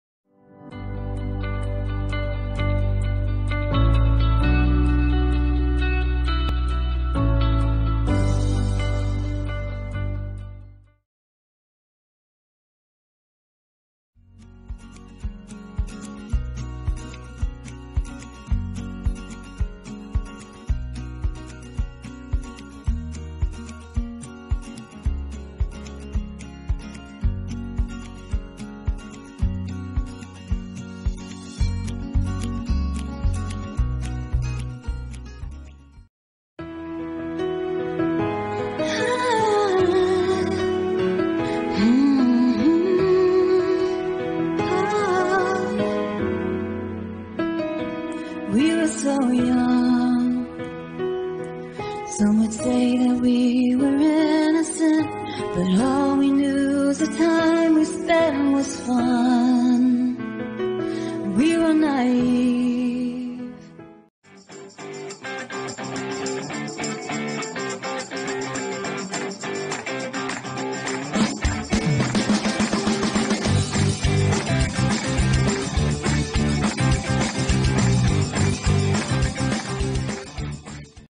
The funny photos of labrador dogs
In this vedio you see watch and feel how cute and fun to have labradors dogs... Love it...